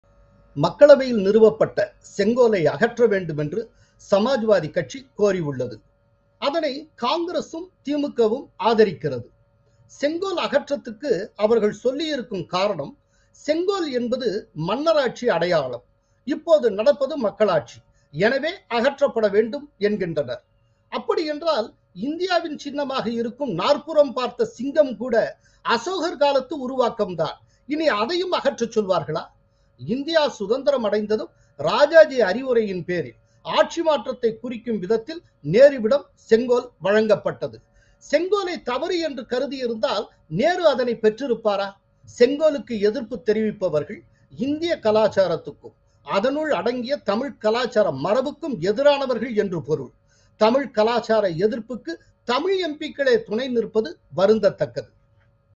செங்கோல் விவகாரம் - தனது கருத்தை பதிவு செய்துள்ளார் மூத்த பத்திரிகையாளர் கோலாகல ஸ்ரீனிவாசன்
Sengol | செங்கோல் விவகாரம் - தனது கருத்தை பதிவு செய்துள்ளார் மூத்த பத்திரிகையாளர் கோலாகல ஸ்ரீனிவாசன்